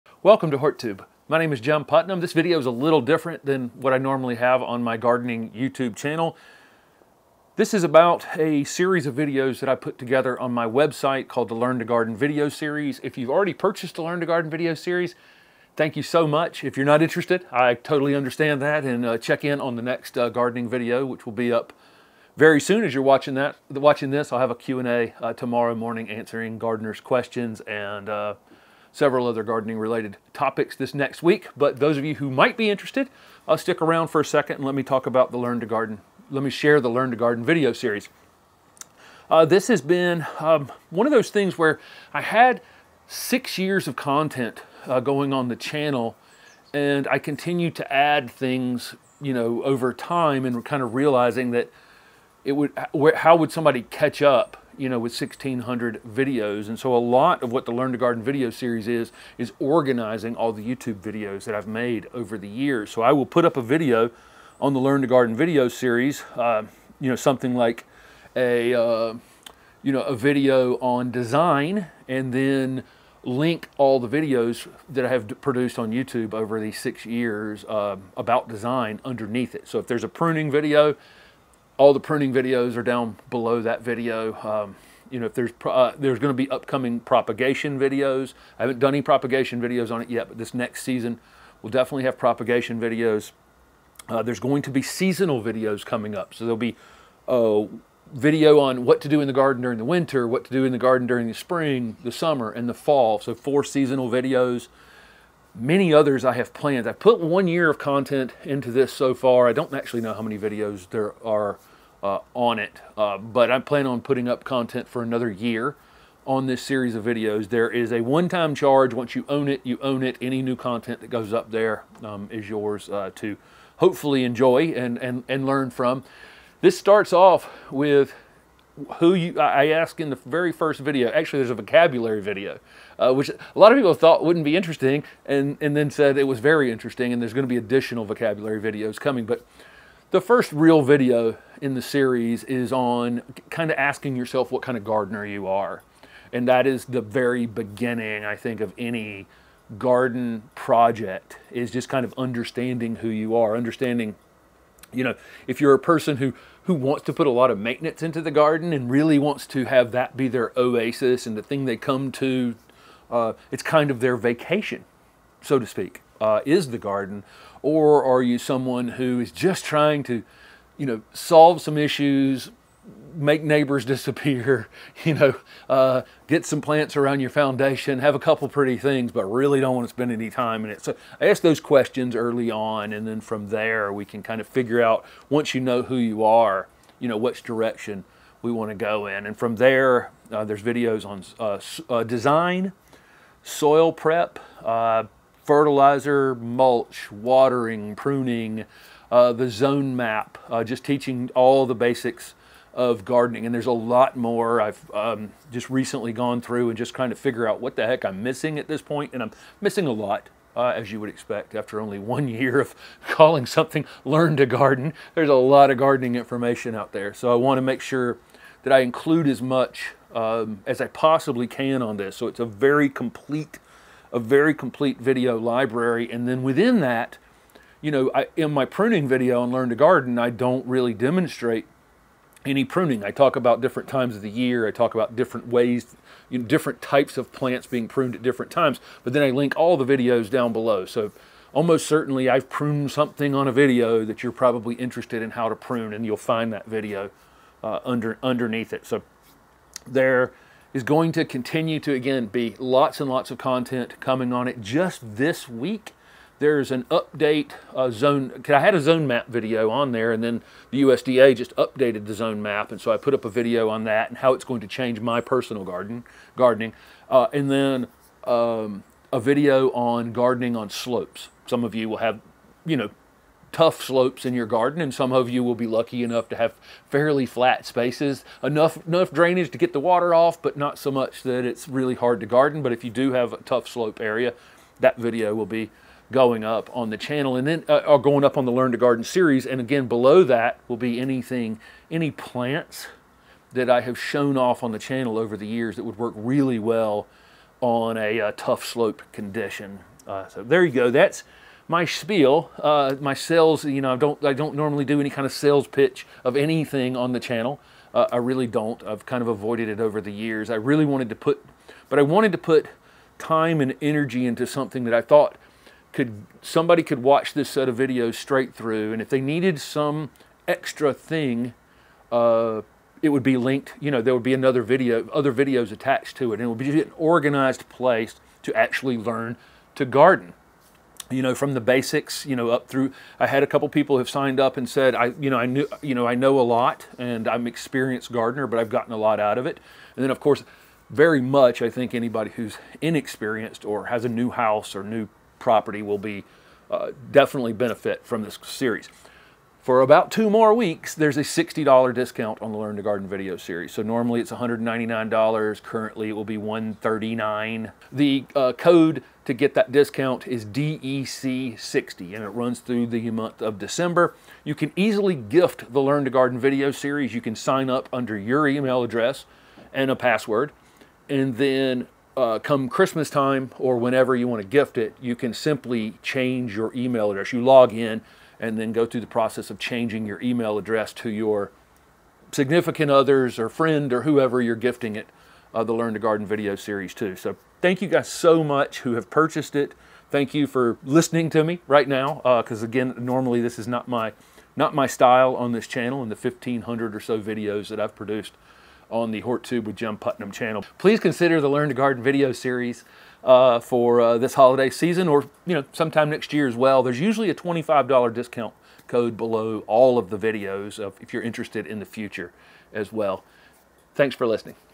Learn to Garden Video Series
Learn to Garden Video Series - In this video I go over details of the learn to garden video series on my website. Horttube.com

Products I Use - Purchases help the channel